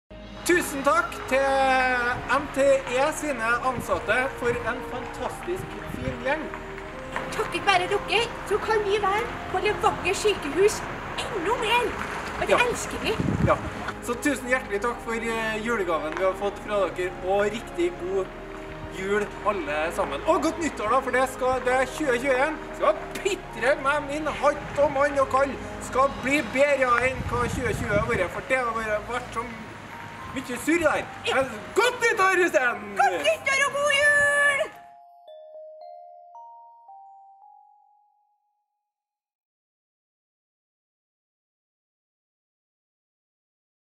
Sykehusklovnene
De ansatte i NTE gir 25 000 kroner til Sykehusklovnene.